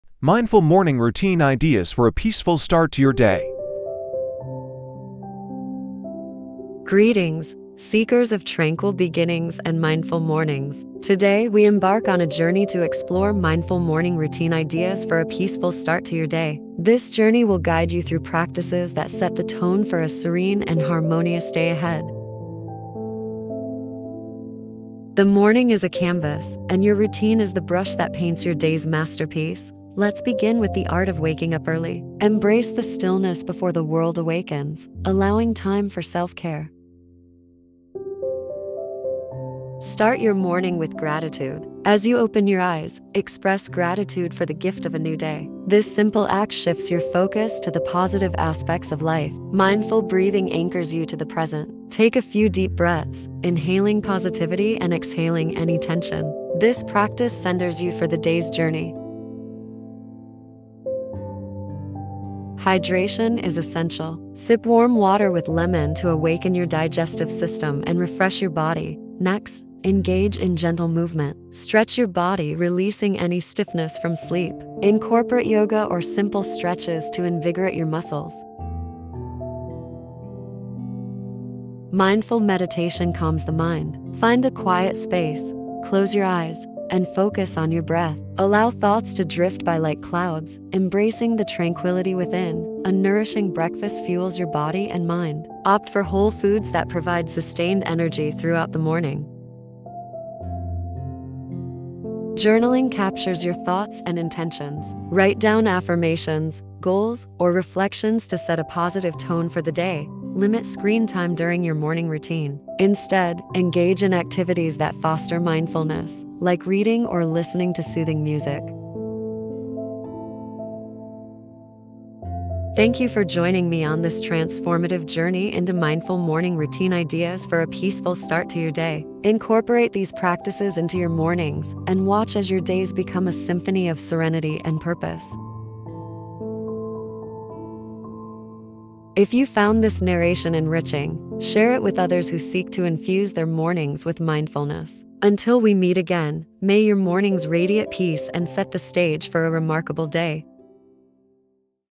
Mindful morning routine ideas for a peaceful start to your day
Elevate your mornings with serenity. Discover "Mindful Morning Routine Ideas for a Peaceful Start to Your Day". Uncover practices that infuse tranquility, mindfulness, and positivity into your morning ritual. Join us to transform your mornings into moments of calm and set the stage for a harmonious and purposeful day ahead.